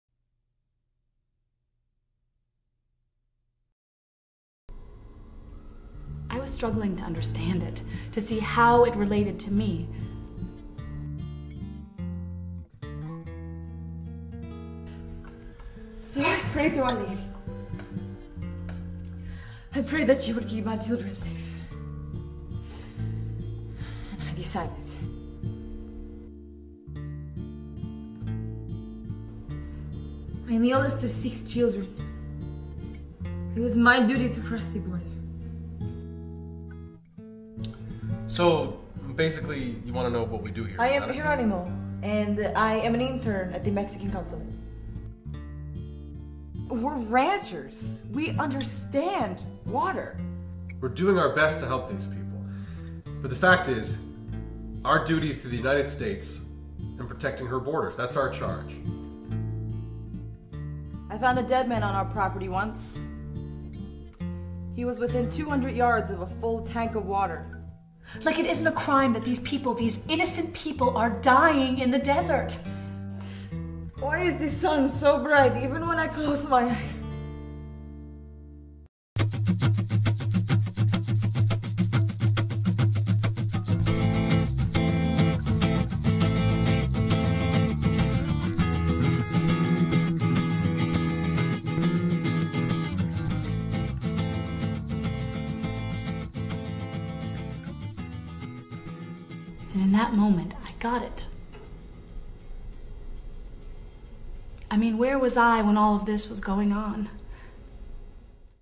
THE LINE IN THE SAND: Stories from the U.S./Mexico Border
THE LINE IN THE SAND: Stories from the U.S./Mexico Border uses the power of theater to tell the personal stories of people affected by U.S./Mexico border migration.

Through an hour-long collection of monologues and photos, audiences are exposed to a variety of points of view on this complex and critical issue.